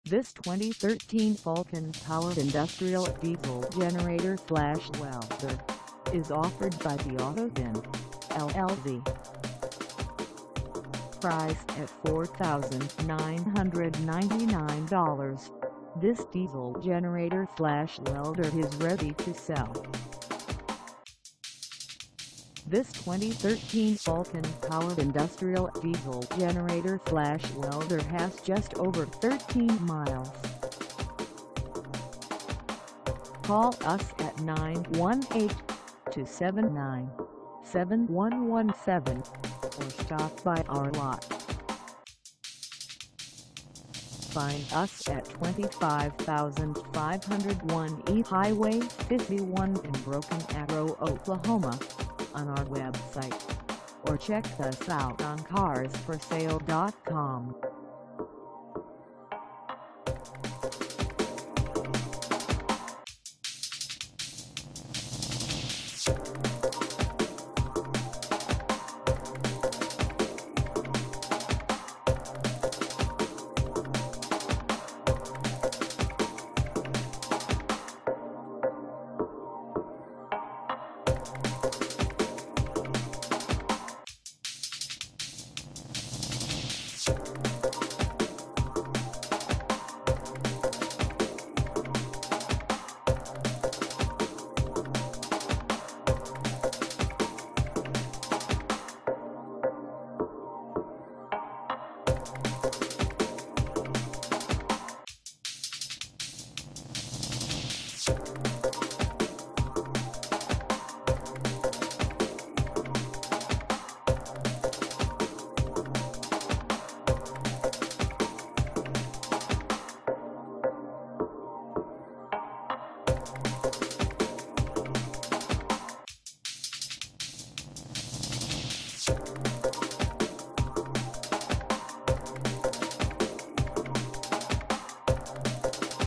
2013 Falcon Power Industrial DIESEL GENERATOR/WELDER 8500ES
THE AUTO BIN, LLC
25501 E. Hwy 51
in Broken Arrow, OK 74014

Come test drive this 2013 Falcon Power Industrial DIESEL GENERATOR/WELDER 8500ES for sale in Broken Arrow, OK.

LIKE BRAND NEW DIESEL GENERATOR/WELDER! ONLY 13MIN ON THIS PERFECT RUNNING MACHAINE!!! WE HAVE BOTH KEYS AND REMOTE START REMOTES AND ALL MANUALS!

WE TOOK THIS IN ON TRADE AND HAVE NO USE FOR IT! WILL TRADE FOR VEHICLES,BIKES QUADS AND OTHER THINGS FOR EQUAL VALUE!

MSRP ON THESE ARE $7,250 THATS $2,250 OFF ON THIS ONE WITH ONLY 13 MINUTES!

A LITTLE DETAILS ON THIS ONE:Current Range of Electric Welding 50-190A Frame Enclosed Silent Frame Fuel Tank Capacity 8 gallons Continuous Run Time 12 hrs Generator Specs:
Frequency 60 Hz Voltage 240/120 Phase Type